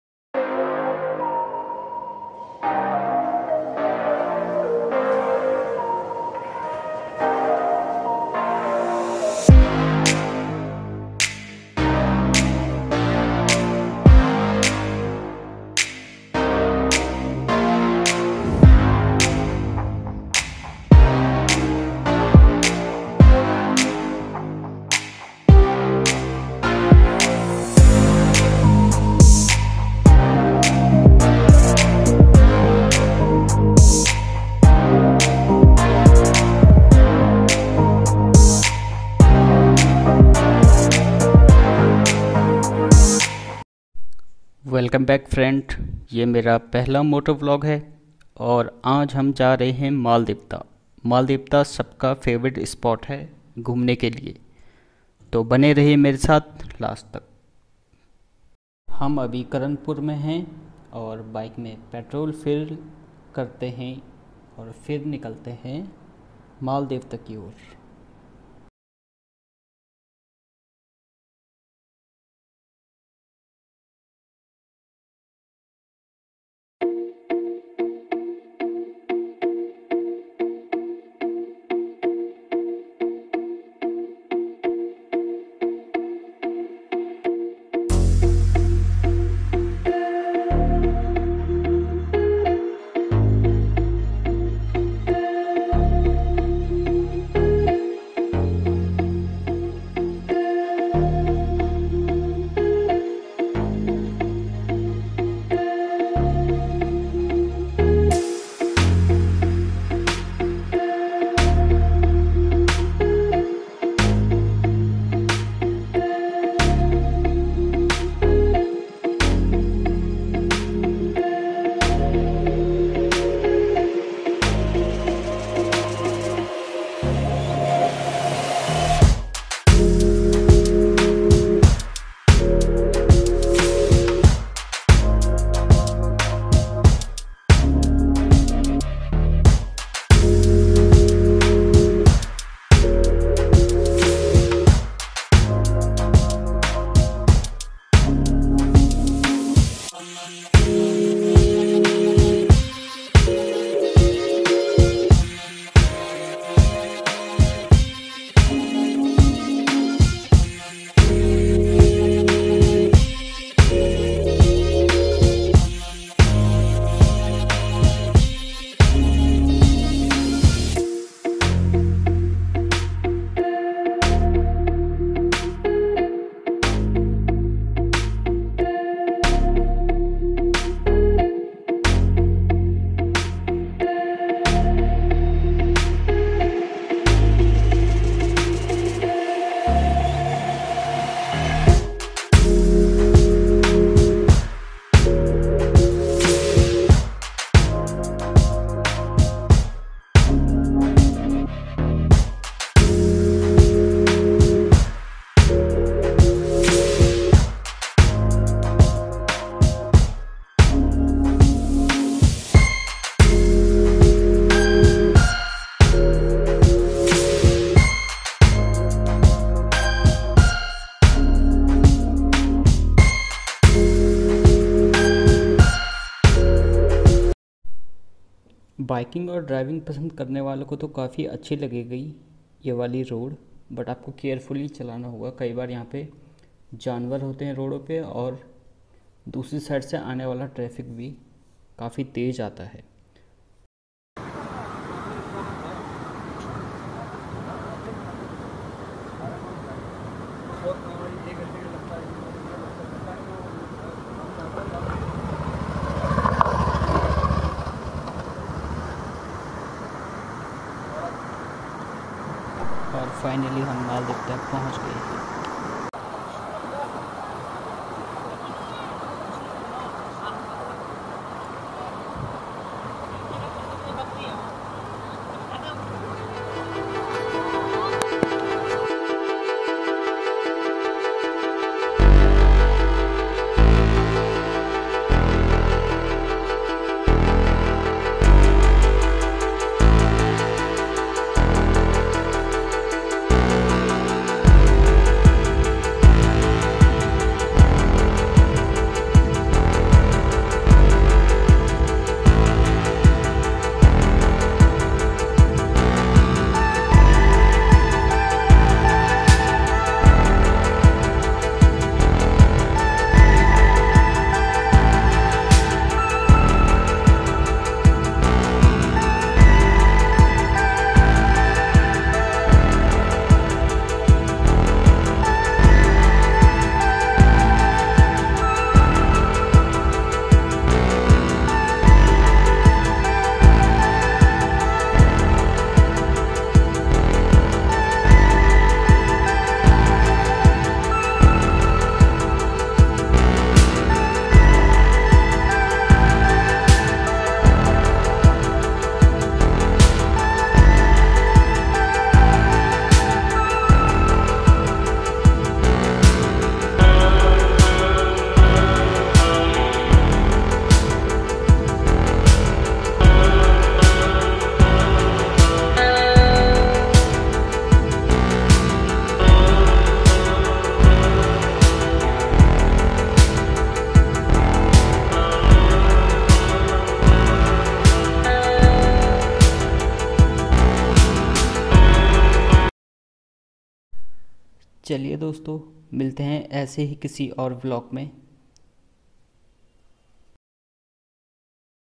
Maldevta || Picknic Spot || Dehradun- Uttarakhand India
Its 20-25 km from main dehradun clock tower. The scenic beauty is amazing. The nearby seasonal river is very beautiful.
Take food and enjoying it at natural place like maldevta.